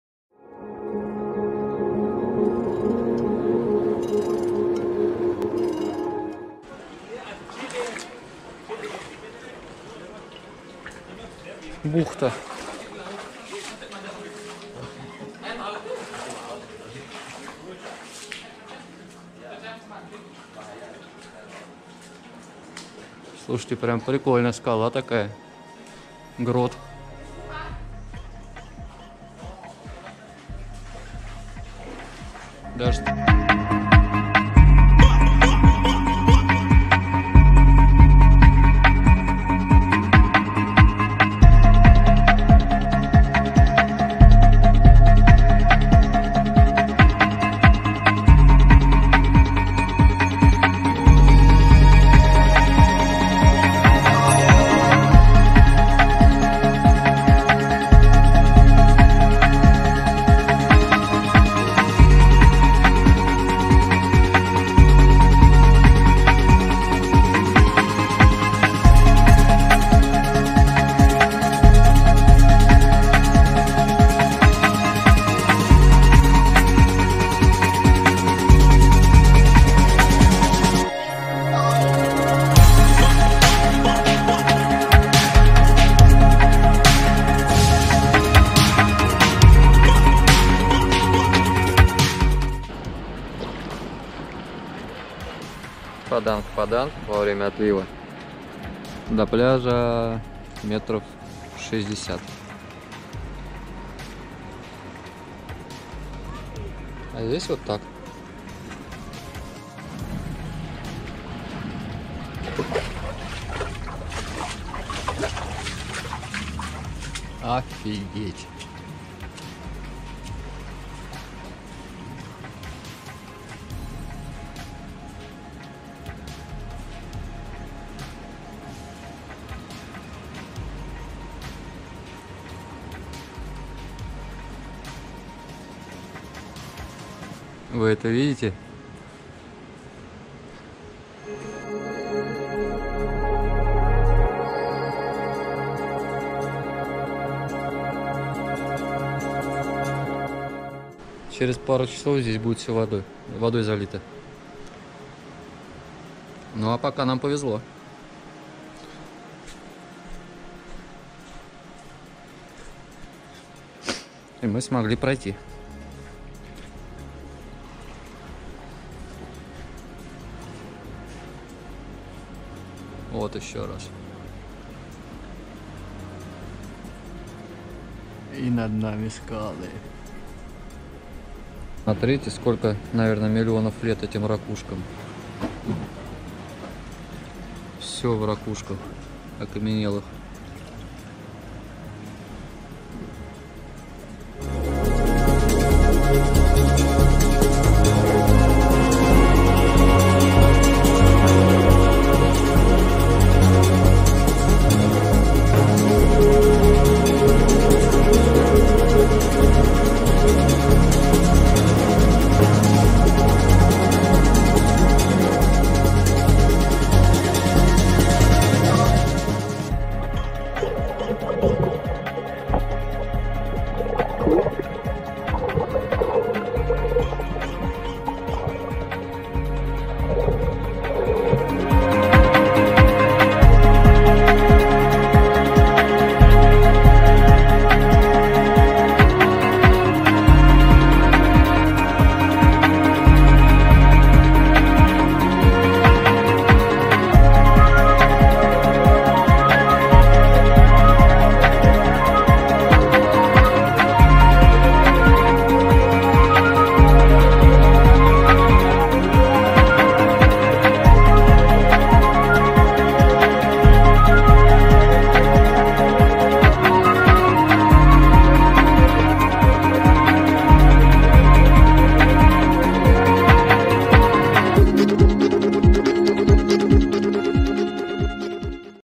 Пляжные истории
Паданг-Паданг (Padang Padang Beach) – популярный среди серферов небольшой пляж на южной окраине Бали, более известный среди местных жителей как Пантай Лабуан Саит. Это живописное побережье в окружении известняковых скал располагается на северо-западе полуостров Букит, по соседству с деревней Пекату и популярным районом Улувату